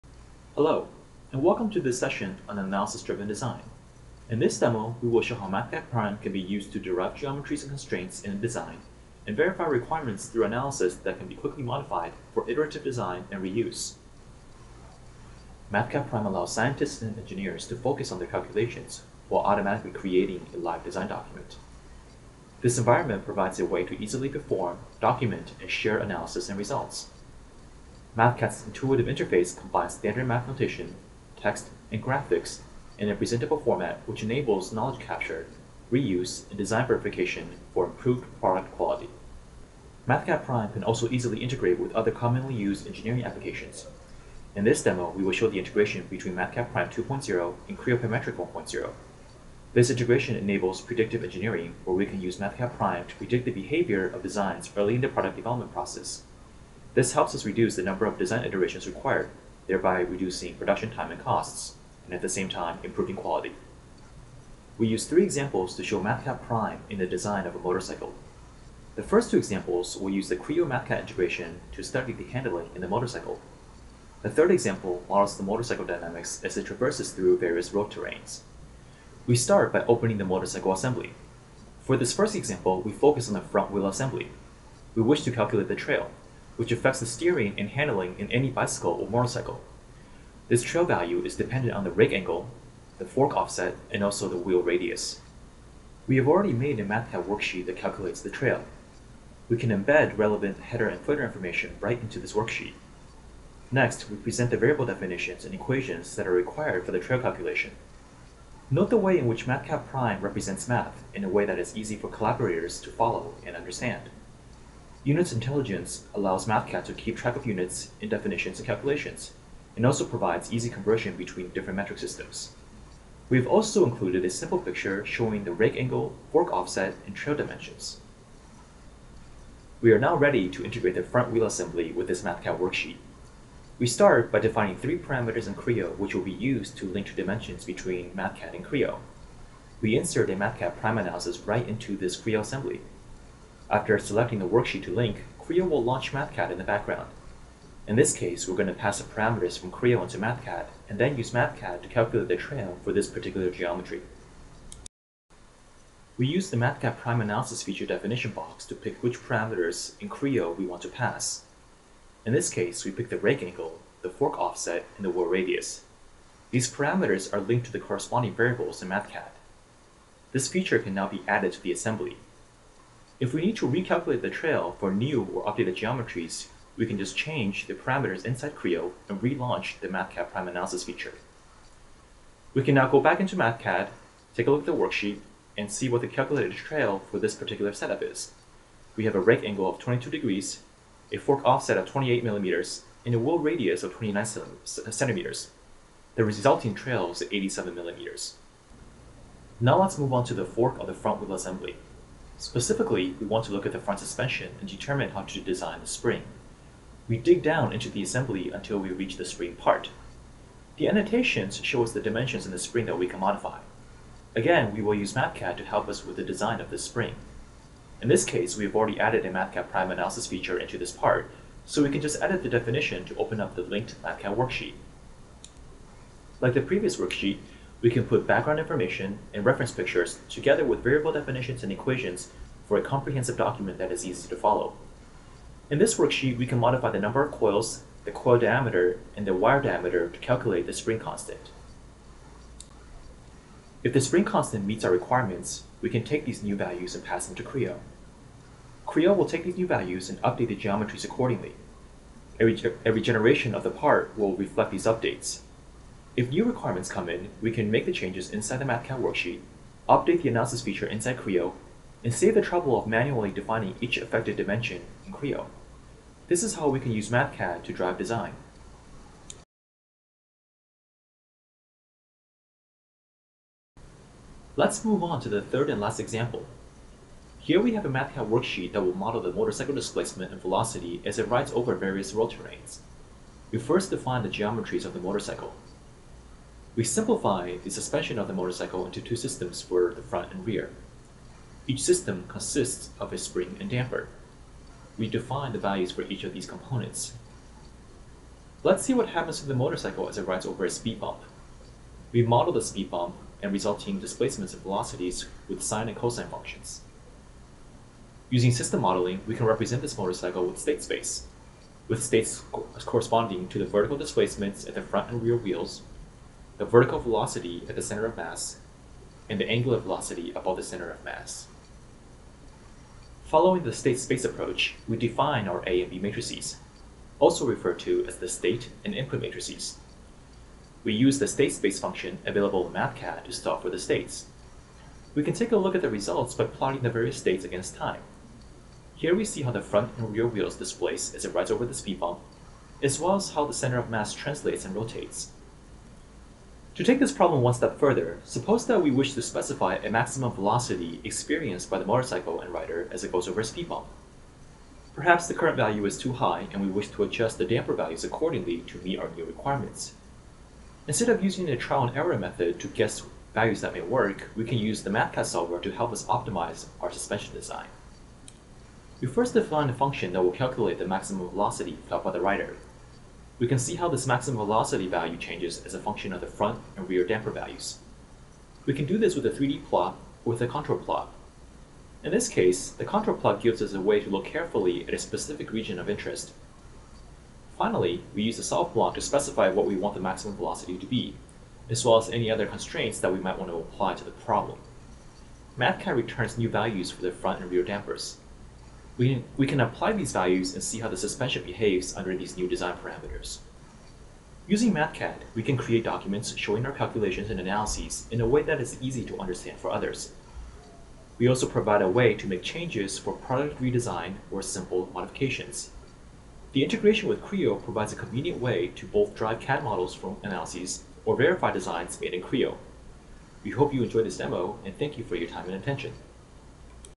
KTM Mathcad Demo
This Demo goes over the interoperability between PTC's Mathcad Prime 2.0 and their 3D CAD solution Creo.